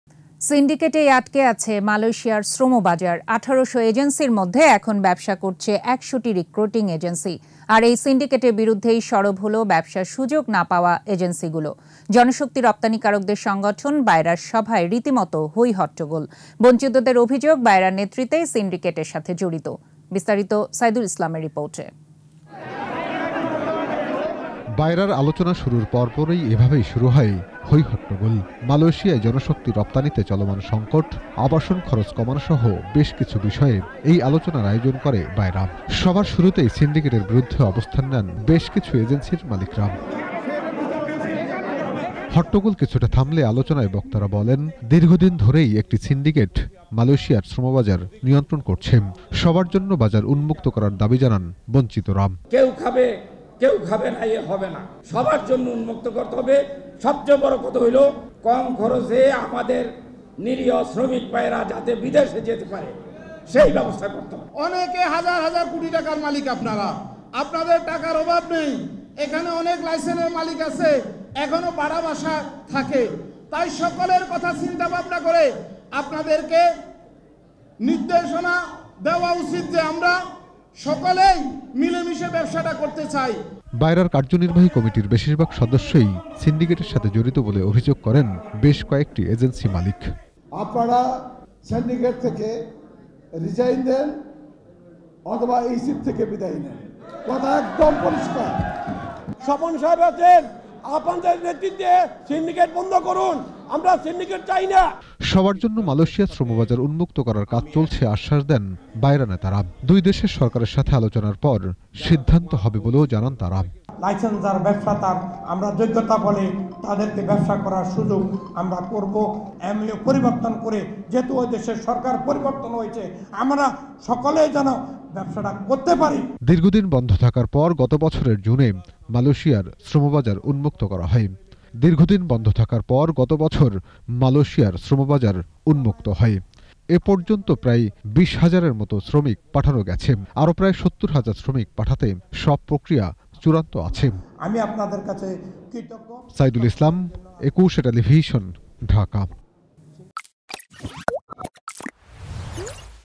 সিন্ডিকেটেই আটকা জনশক্তি রফতানি, সরব বঞ্চিতরা | ETV News
সিন্ডিকেটেই আটকে আছে মালয়েশিয়ার শ্রমবাজার। ১৮শ’ এজেন্সির মধ্যে এখন ব্যবসা করছে একশ’টি রিক্রুটিং এজেন্সি। আর এই সিন্ডিকেটের বিরুদ্ধেই সরব হলো ব্যবসার সুযোগ না পাওয়া এজেন্সিগুলো। জনশক্তি রপ্তানিকারকদের সংগঠন বায়রার সভায় রীতিমতো হৈ হট্টগোল। বঞ্চিতদের অভিযোগ, বায়রার নেতৃত্বই সিন্ডিকেটের সাথে জড়িত। বিস্তারিত সাইদুল ইসলামের রিপোর্টে।